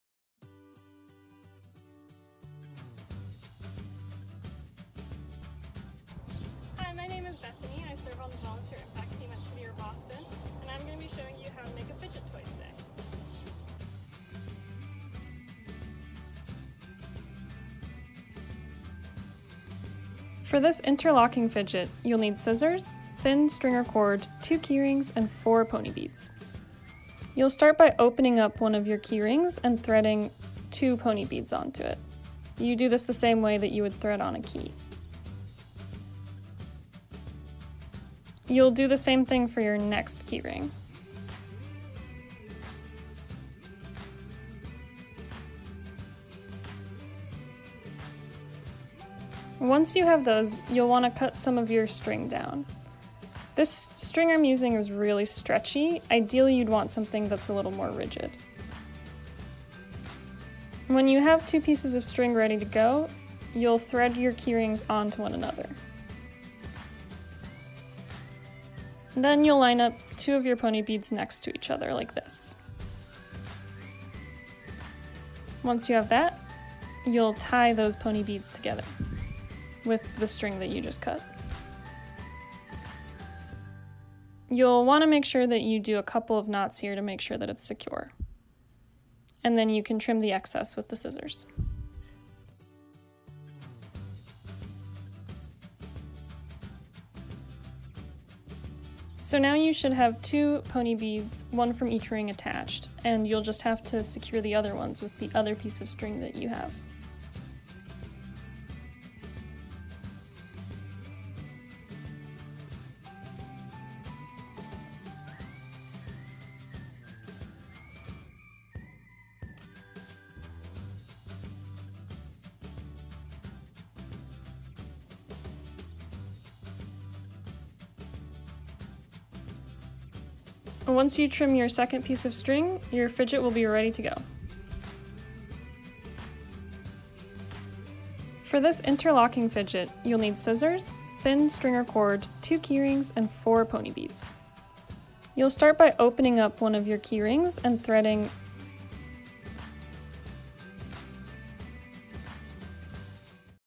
DIY Interlocking Fidget
Follow along with Bethany and learn how to make an easy Interlocking Fidget toy